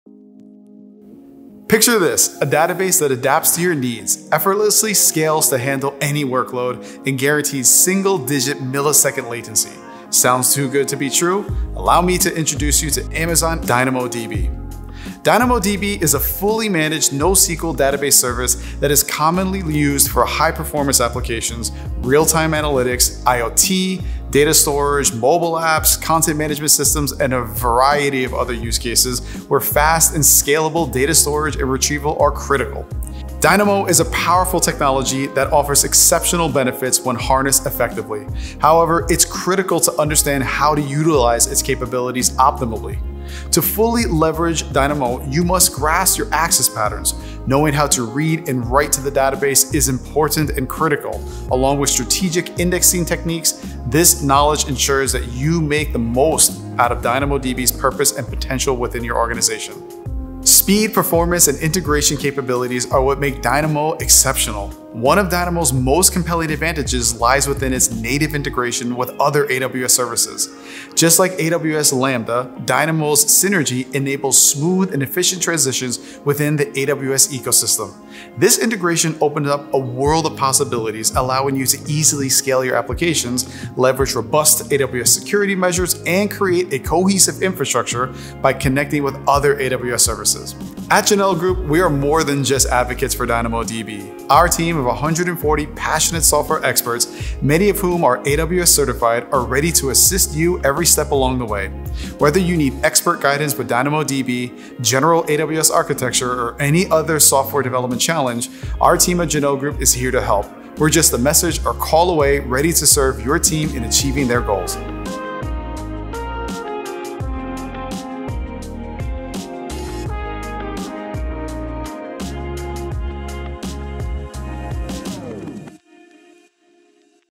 DynamoDB vs. MongoDB: Why Amazon's DynamoDB is Leading the Future of Databases
In the dynamic landscape of technology, choosing the right database is paramount. In this deep dive, we contrast Amazon DynamoDB with databases like MongoDB to unveil why DynamoDB stands out. Discover DynamoDB's unique integrations with AWS services like Lambda, S3, and IAM, making for scalable, secure, and efficient applications. From access patterns to strategic indexing, let's unlock the true power of DynamoDB for your projects.

Presented by Steven Zgaljic, CTO of Jahnel Group, Inc.